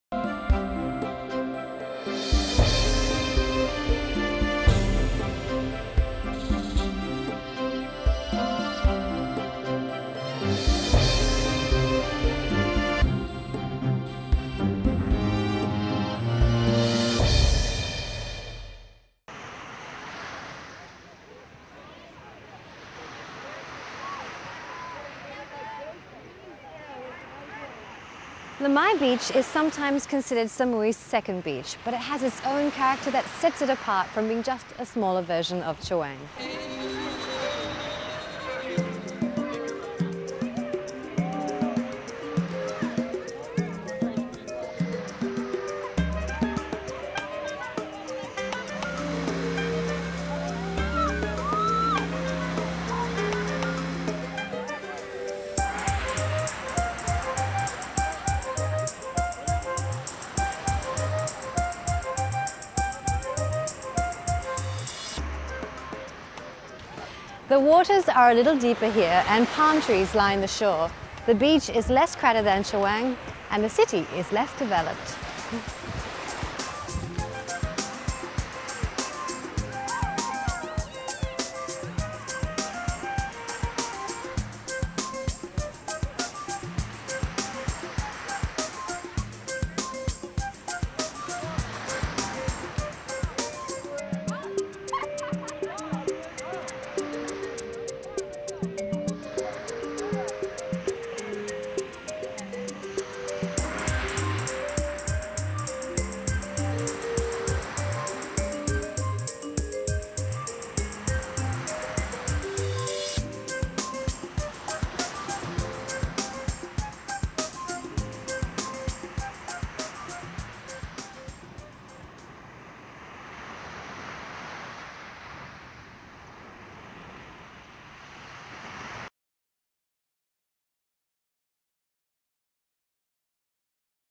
Vivien Tan, Lamai Beach Inside Guide - Koh Samui
Inside Guide Koh Samui, hosted by Vivien Tan.  We visit Lamai Beach on the beautiful Thai island of Koh Samui. Produced by Lanna Studio.